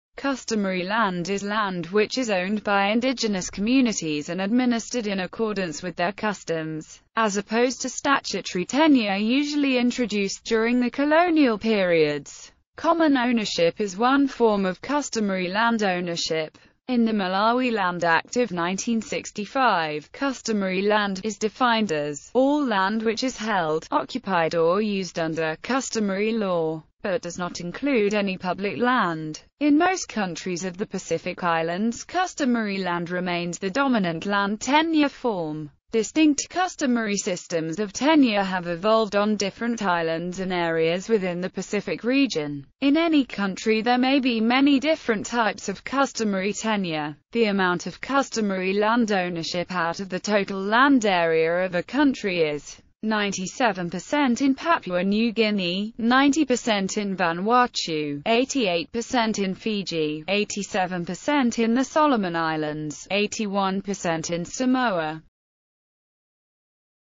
Customary land
Customary land is land which is owned by Indigenous communities and administered in accordance with their customs, as opposed to statutory tenure usually introduced during the colonial periods.Common ownership is one form of customary land ownership.